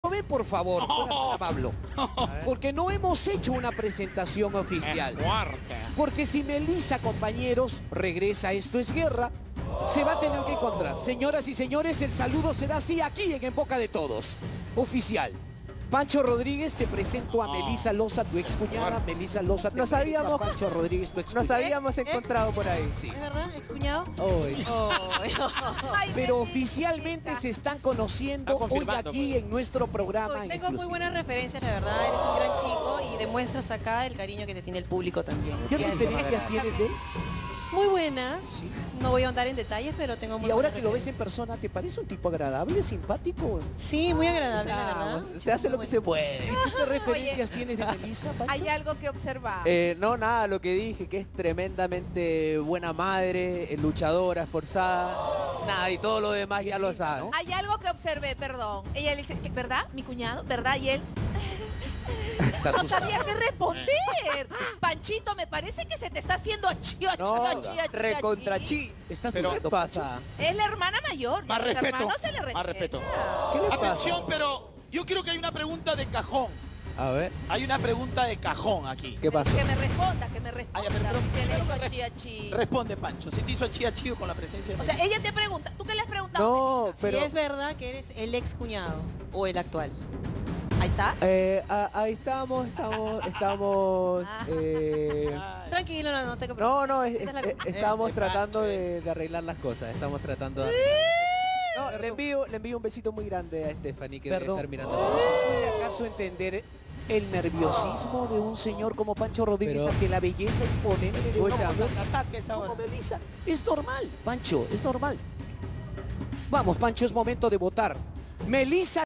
Melissa Loza puso nervioso a Pancho Rodríguez con pregunta sobre su hermana Spheffany
Melissa Loza se reencontró con Pancho Rodríguez y fue más directa que nunca.